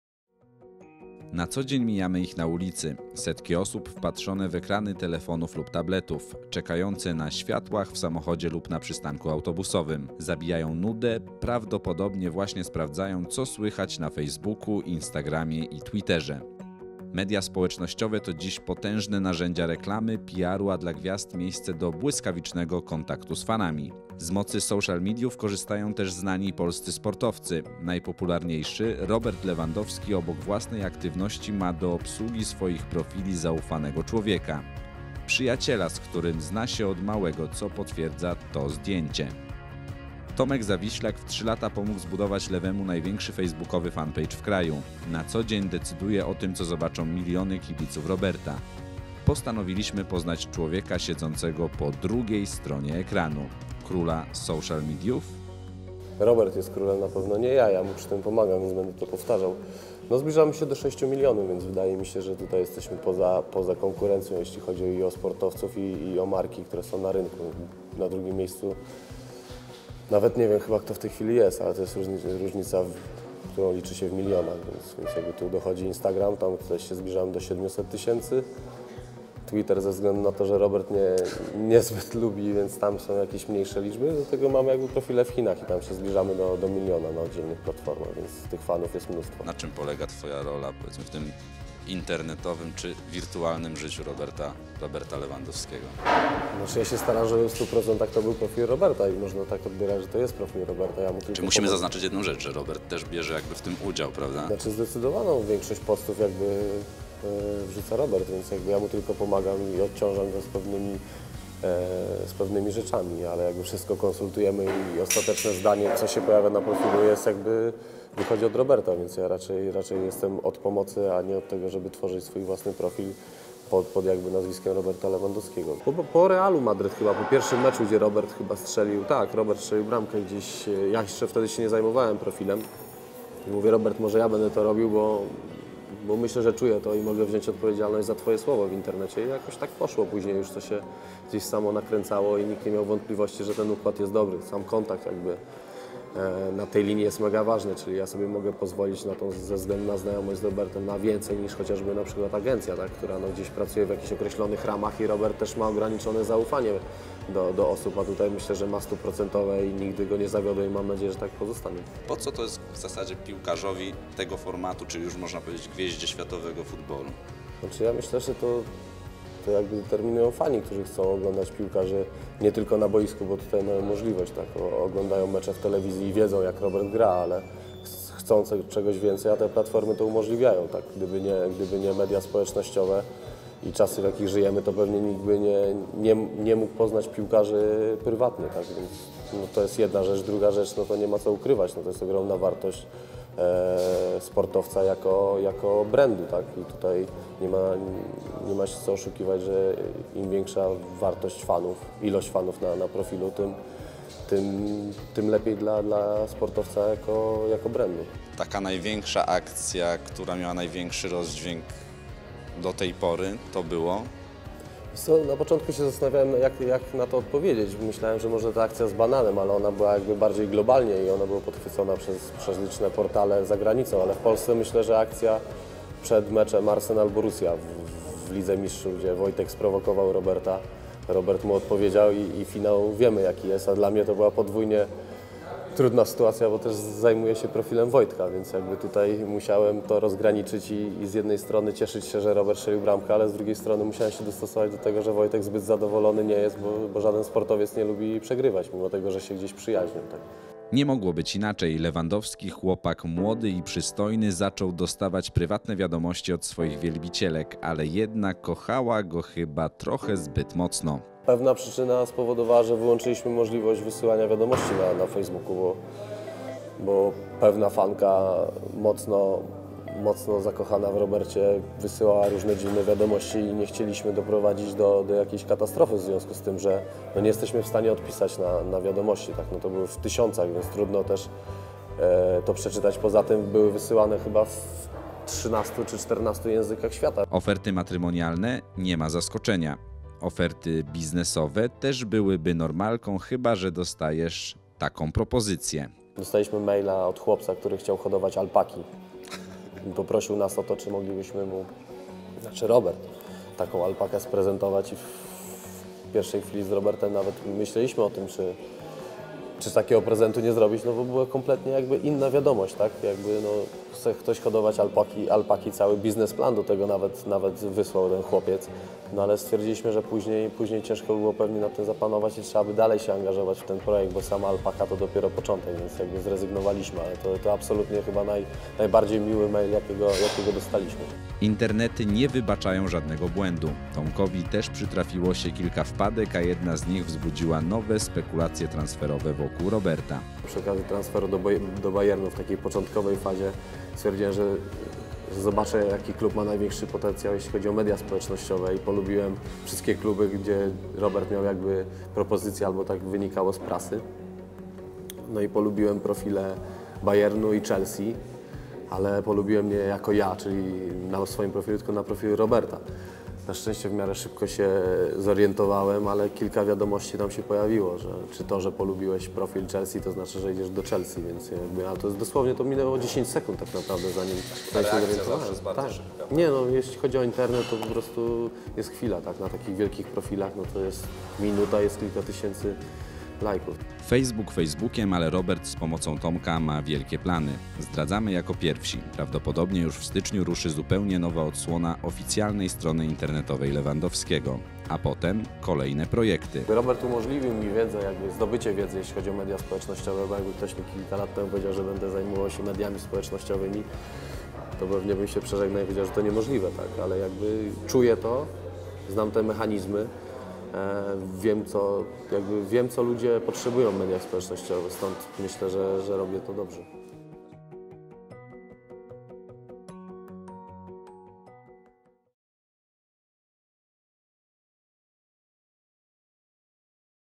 Zobacz kto prowadzi profil Lewandowskiego na Facebooku
– W tej chwili w kraju jesteśmy poza konkurencją – mówi Tomasz Zawiślak, wieloletni przyjaciel Roberta Lewandowskiego, który zarządza jego aktywnością w sieci. Napastnik Bayernu Monachium ma największy fanpage w Polsce – blisko sześć milionów fanów.